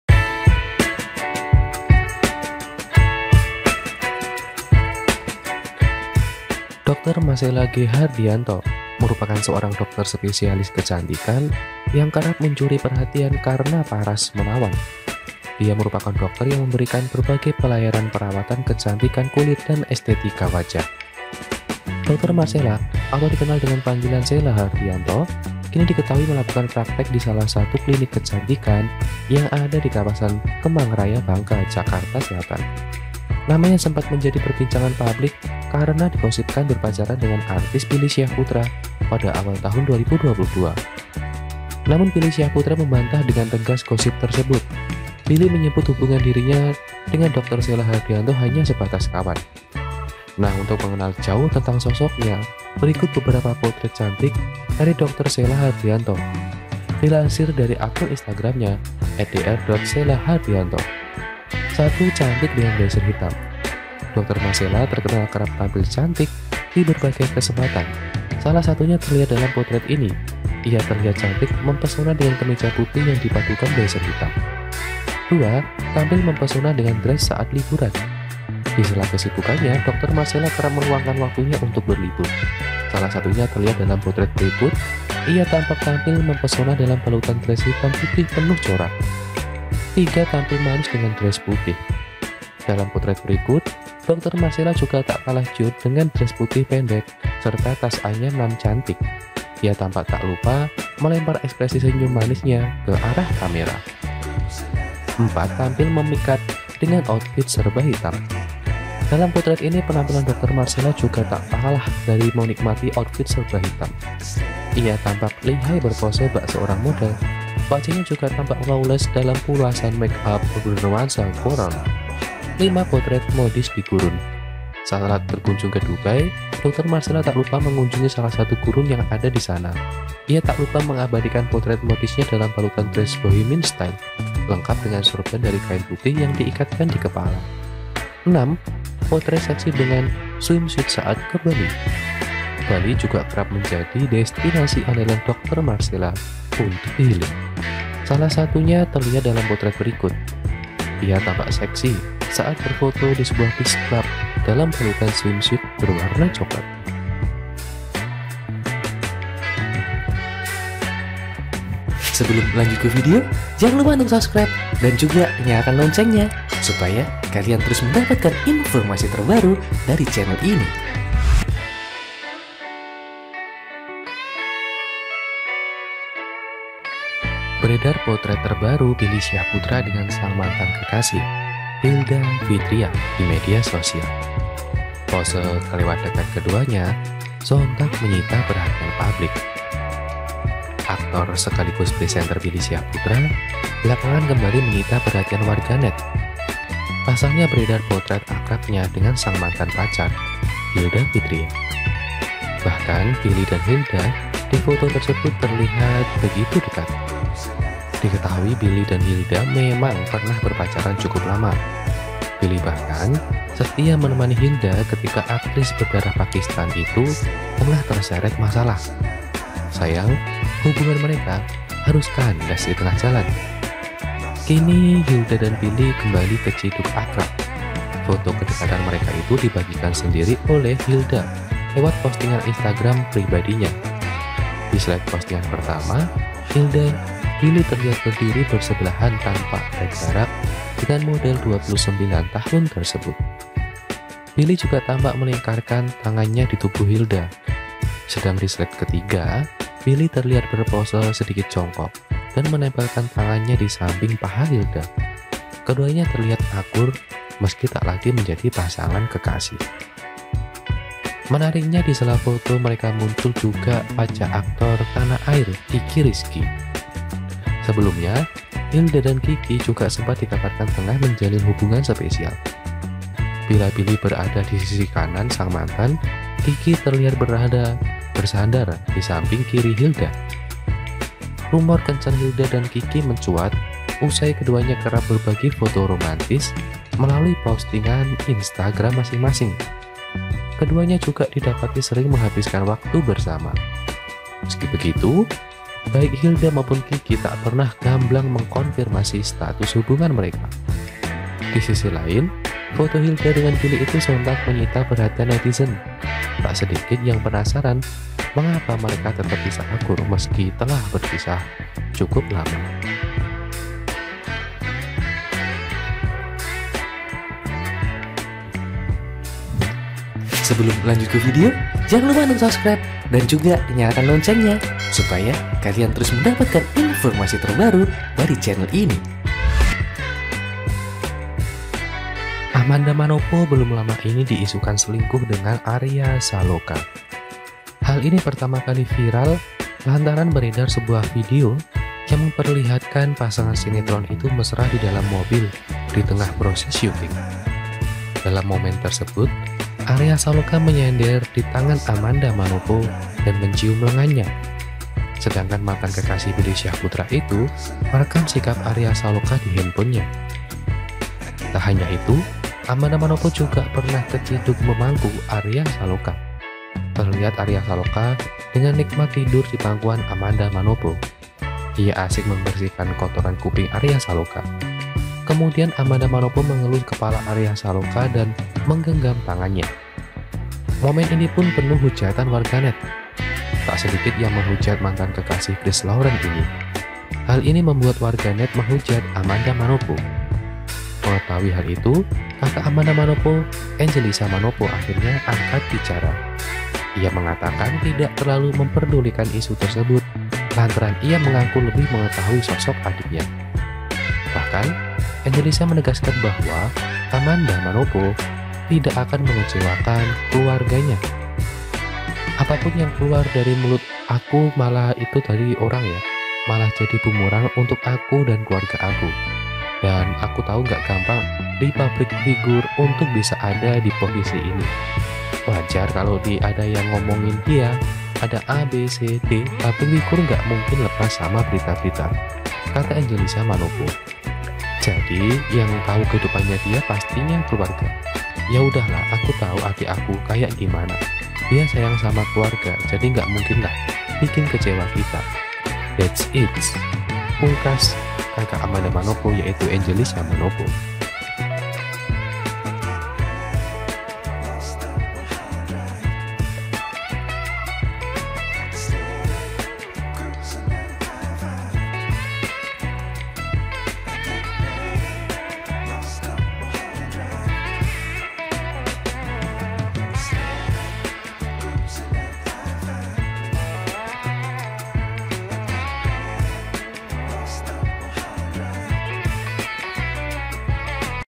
DIGOSIPKAN BILLY PUNYA PACAR BARU ❗❓ TERCYDUK DI CLUB MALAM BARENG DOKTER CANTIK INI
6 Potret Dokter Cantik Shella Hardianto, Sempat Digosipkan Pacaran dengan Billy Syahputra!

Wiwie Heriyani, MNC Portal · Kamis 08 Juni 2023 13:00 WIB

Marshella G Hardianto merupakan seorang dokter Spesialis Kecantikan yang kerap mencuri perhatian karena paras menawa. Dia merupakan doter yang memberikan berbagai pelayanan perawatan kecantikan kulit dan estetika wajah.

Dokter Marshella atau dikenal dengan panggilan Shella Hardianto, kini diketahui melakukan praktek di salah satu klinik kecantikan yang ada di kawasan Kemang Raya Bangka, Jakarta Selatan. Namanya sempat menjadi perbincangan publik karena digosipkan berpacaran dengan artis Billy Syahputra pada awal tahun 2022.
Namun, Billy Syahputra membantah dengan tegas gosip tersebut. Billy menyebut hubungan dirinya dan Dokter Shella Hardianto hanya sebatas kawan.

Nah, untuk mengenal jauh tentang sosoknya, berikut beberapa potret cantik dari Dokter Shella Hardianto, dilansir dari akun Instagramnya, @dr.shellahardianto.

1. Cantik dengan blazer hitam

Dokter Marshella terkenal kerap tampil cantik di berbagai kesempatan. Salah satunya terlihat dalam potret ini. Ia terlihat cantik memesona dengan kemeja putih yang dipadukan blazer hitam.

2. Tampil memesona dengan dres saat liburan

Di sela kesibukannya, Dokter Marshella kerap meluangkan waktunya untuk berlibur. Salah satunya terlihat dalam potret berikut. Ia tampak tampil memesona dalam balutan dress hitam putih penuh corak.

3. Tampil manis dengan dres putih

Dalam potret berikut, Dokter Marshella juga tak kalah cute dengan dress putih pendek serta tas anyam nan cantik. Ia tampak tak lupa melempar ekspresi senyum manisnya ke arah kamera.

4. Tampil memikat dengan outfit serba hitam

Dalam potret ini, penampilan Dokter Marshella juga tampak tak kalah memikat dengan outfit serha hitam. Ia tampak lihai berpose bak seorang model. Wajahnya juga tampak flawless dalam pulasan make up bernuansa coral.

5. Potret modis di gurun

Saat berkunjung ke Dubai, Dokter Marshella tak lupa mengunjungi salah satu gurun yang ada disana. Ia tak lupa mengabadikan potret modisnya dalam balutan dres bohemian style, lengkap dengan sorban dari kain putih yang diikatkan di kepalanya.

6. Potret seksi dengan swimsuit saat ke Bali

Bali juga kerap menjadi destinasi andalan Dokter Marshella untuk healing. Salah satunya terlihat dalam potret berikut. Ia tampak seksi saat berfoto di sebuah beach club dalam balutan swimsuit berwarna coklat.